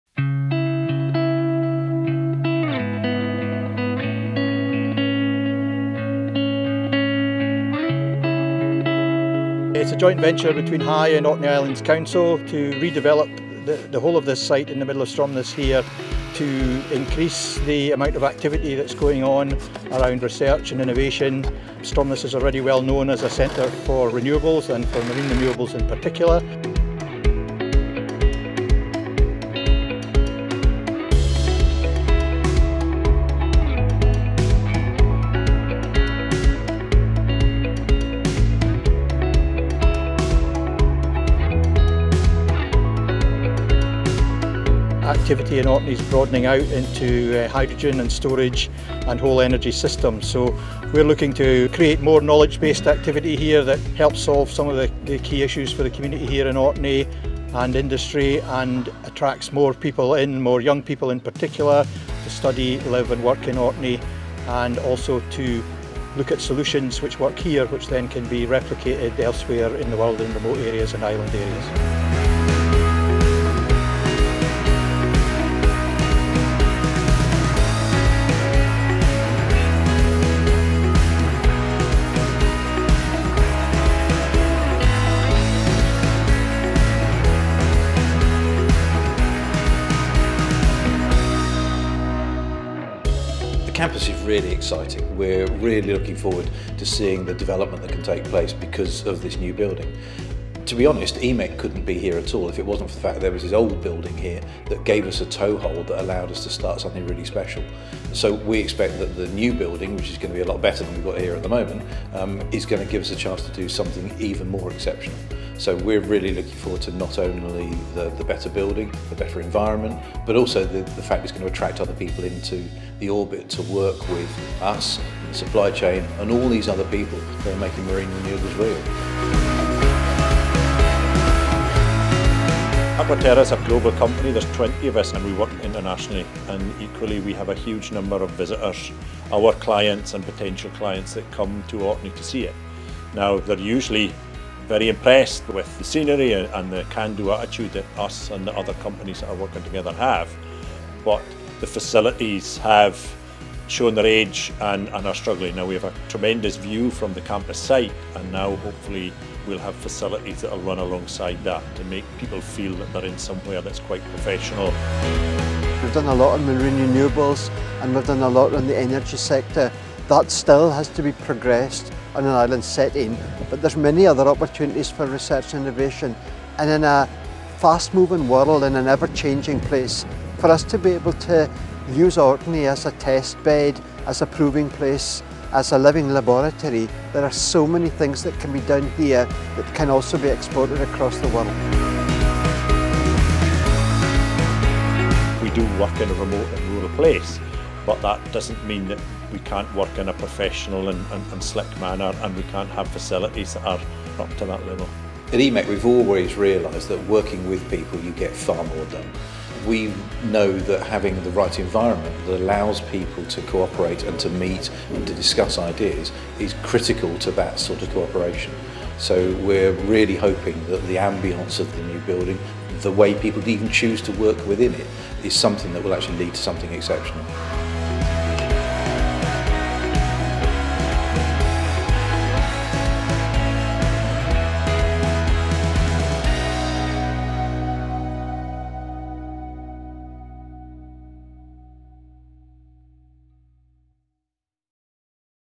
New campus to cement Orkney's reputation as islands of innovation
A multi-million pound project to create a new home for Orkney's wide range of energy expertise is beginning to take shape in Stromness.

Orkney is at the forefront of ground-breaking work in marine energy and pioneering renewables research, and the Orkney Research and Innovation Campus will help to build upon this work.